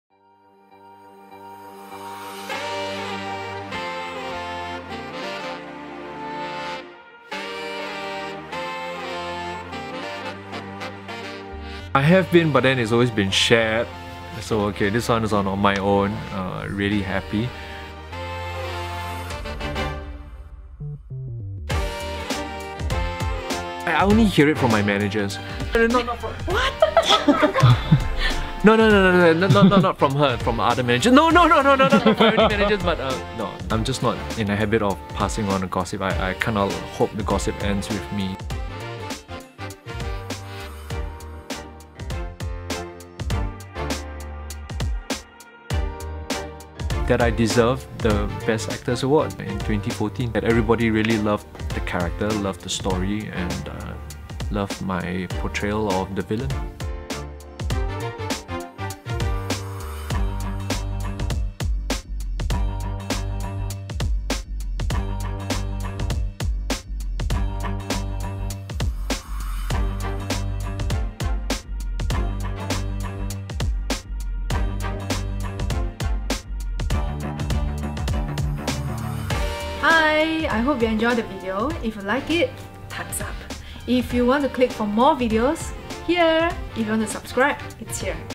Pierre Png's main source of gossip is... (Behind The Scenes of his I-weekly Cover Shoot)
What a hunk! Check out Pierre's first solo cover on I-weekly! Also, he shares with us his gossip source!